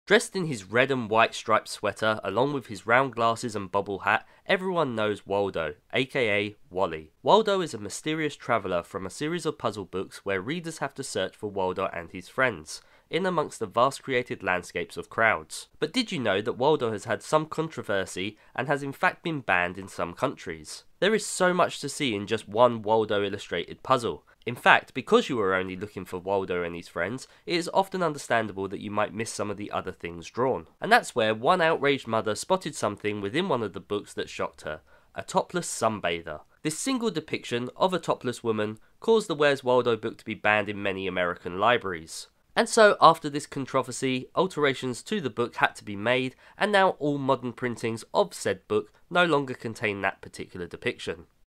BANNED! - Where's Waldo Children's Book
Where's Waldo aka Where's Wally - Banned in Various American Libraries!